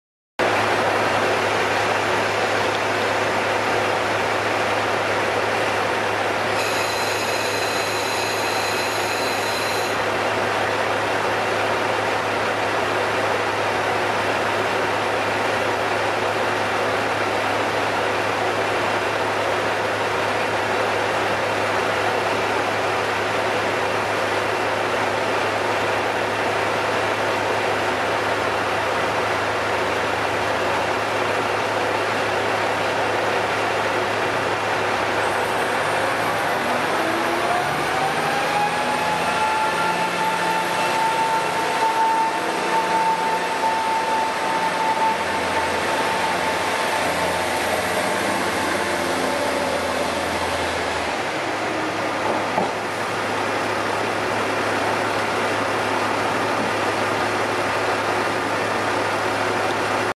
磐越西線キハ48形回送 会津若松駅発車 JR East Ban’etsu West Line KiHa48 series DMU
新潟色キハ40系気動車2両編成の回送列車が2番線から出発する。
East Japan Railway Banetsu West Line deadhead train will depart from Aizu-Wakamatsu station in Fukushima Prefecture.
東日本旅客鐵道磐越西線KiHa48型柴聯車將離開會津若松車站。
동일본 여객철도 반에쓰사이 선 열차는 아이즈와카마쓰 역에서 출발합니다.